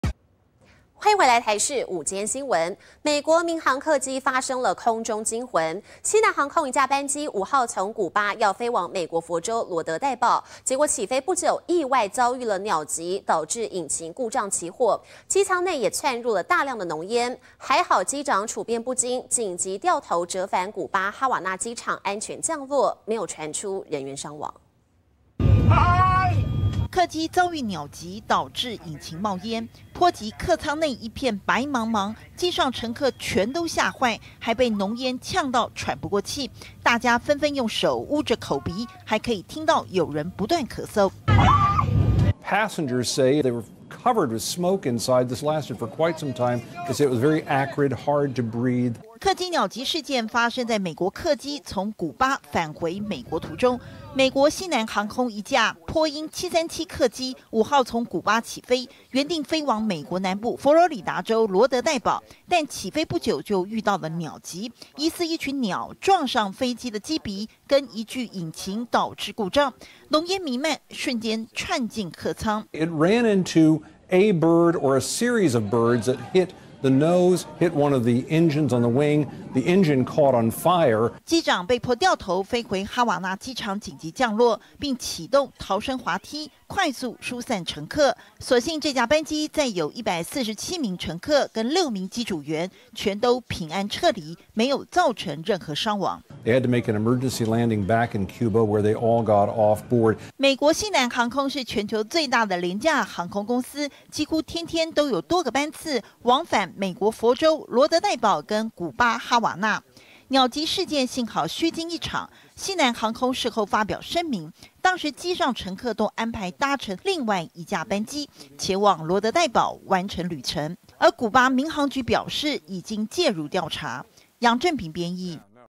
西南航空遇鳥擊！ 濃煙竄艙內急折返哈瓦那
美國民航客機發生空中驚魂，美國西南航空一架班機，5號從古巴要飛往美國佛州「羅德岱堡」途中，不料起飛不久意外遭遇鳥擊，導致引擎故障起火，機艙內也充滿大量濃煙，幸好機長處變不驚，緊急掉頭返回古巴哈瓦那機場安全降落，沒有傳出人員傳出。